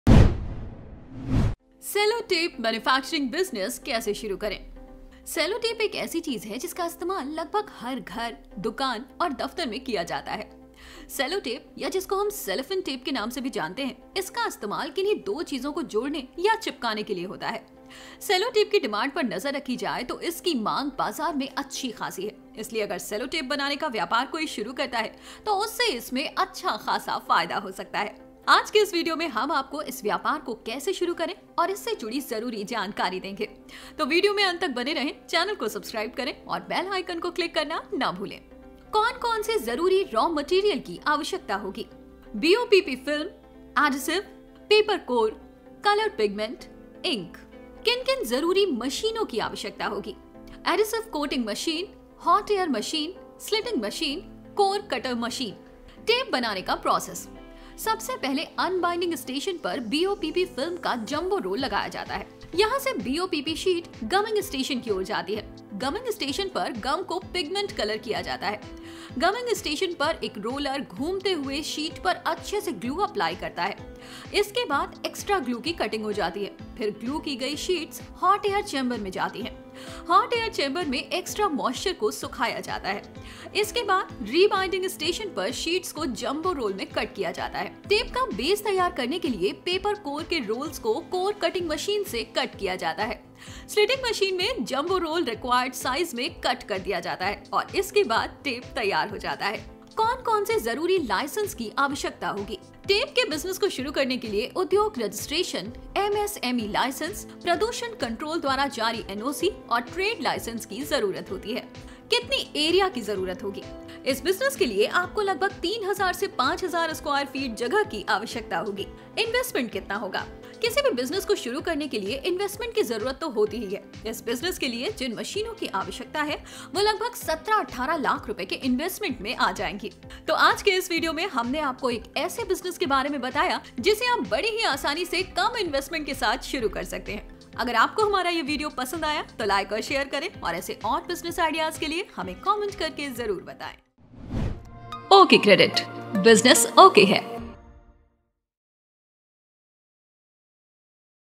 Cellotape Manufacturing Business | सेलो टेप मैन्युफैक्चरिंग बिजनेस | OkCredit
सेलो टेप एक ऐसी चीज़ है, जिसका इस्तेमाल लगभग हर घर, दुकान और दफ्तर में किया जाता है। सेलो टेप या जिसको हम सेलोफ़िन टेप के नाम से भी जानते हैं। इसका इस्तेमाल किन्ही दो चीजों को जोड़ने या चिपकाने के लिए होता है।सेलो टेप की डिमांड पर नजर डाली जाए, तो इसकी मांग बाजार में अच्छी खासी है। इसलिए अगर सेलो टेप बनाने का व्यापार कोई शुरू करता है, तो उसे इसमें अच्छा खासा फायदा हो सकता है।आज के इस वीडियो में हम आपको इस व्यापार को कैसे शुरू करें और इससे जुड़ी ज़रूरी जानकारी देंगे।